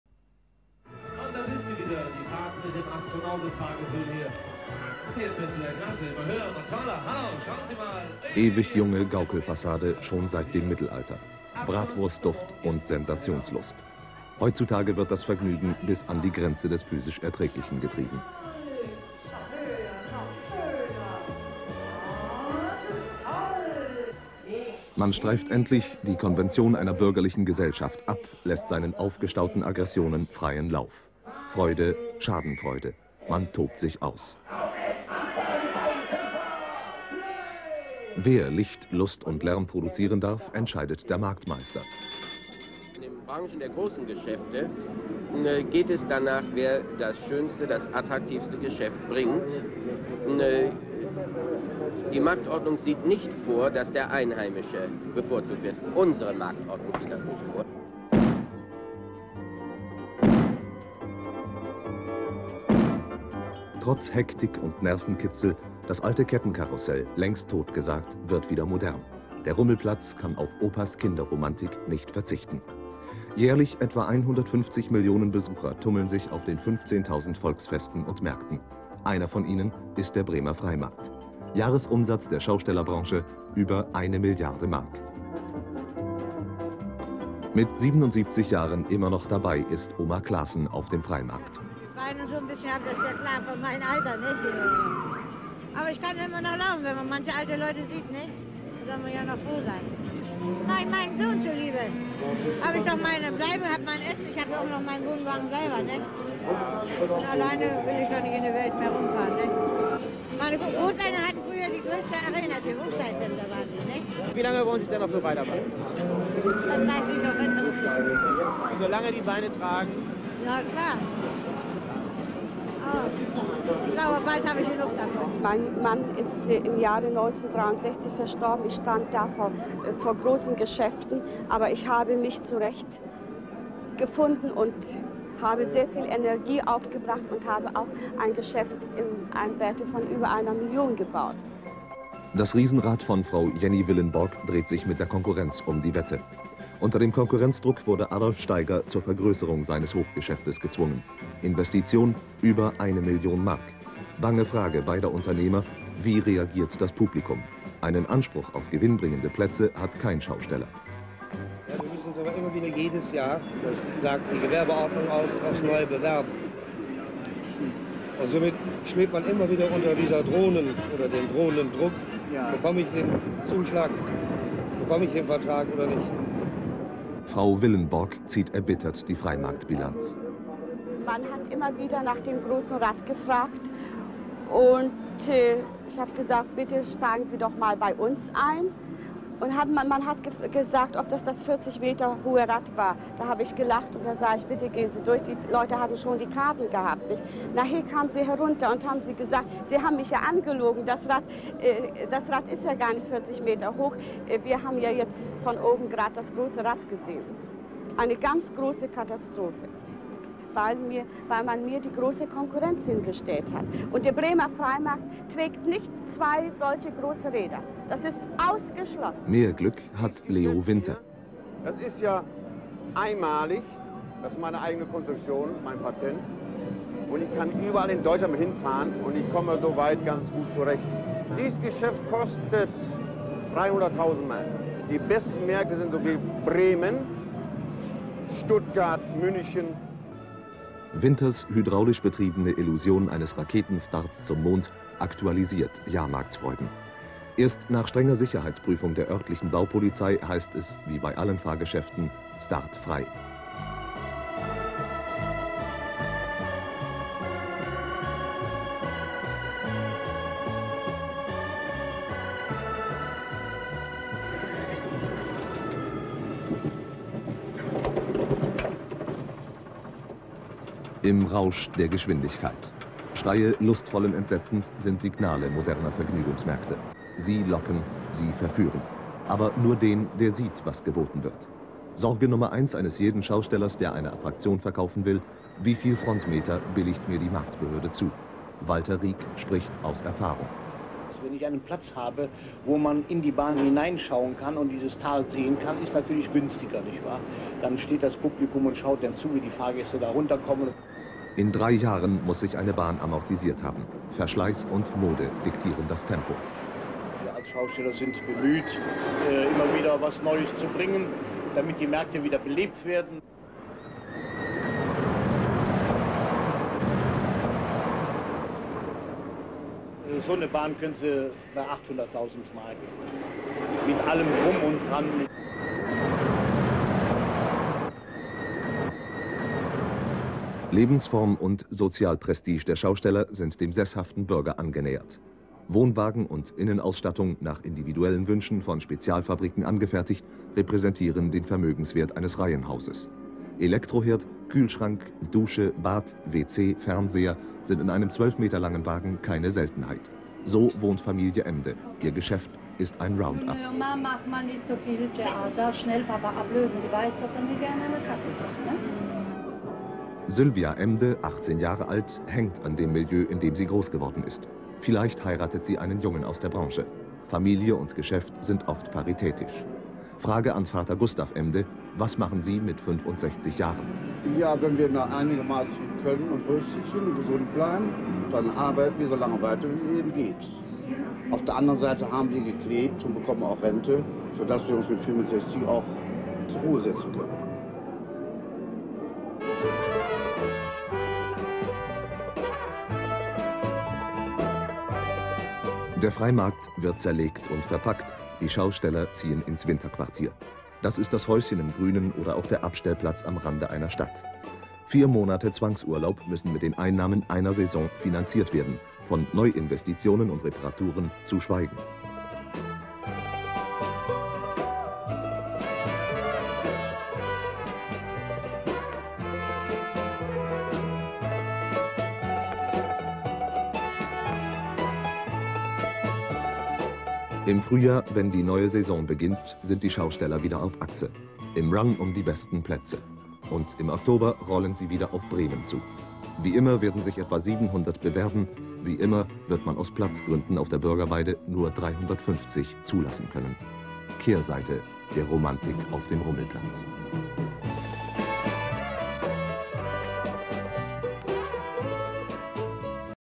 Freimarkt1968/1970. Marktmeister, Stellplätze, Investitionen und Rivalität auf dem Freimarkt.
Marktmeister, Stellplätze, Investitionen und Rivalität auf dem Freimarkt.